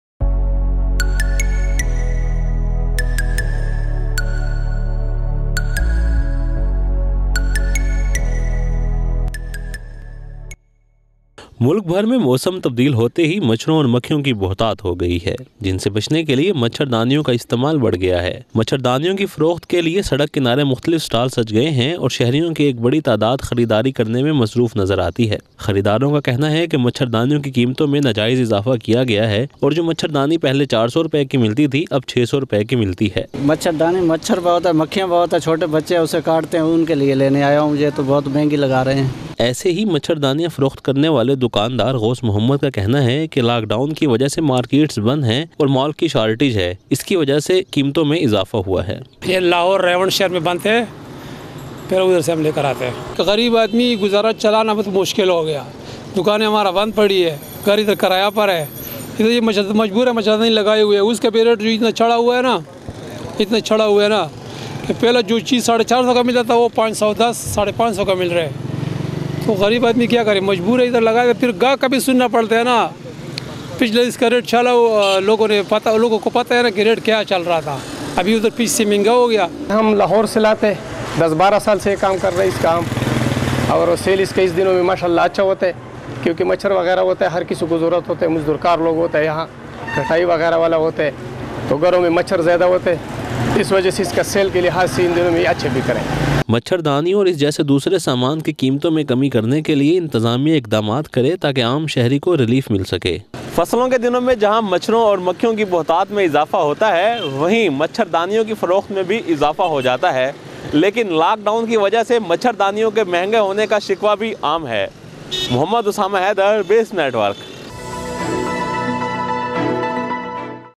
mosquito net on roads sale | BISM Network
As the weather changes across the country, the number of mosquitoes and flies has increased, which has increased the use of mosquitoes to protect them. Various street stalls have been decorated for sale of mosquitoes where a large number of citizens appear to be busy shopping. Buyers say that the prices of mosquito nets have been increased illegally and the mosquito net used to be Rs 400 is now available at Rs 600.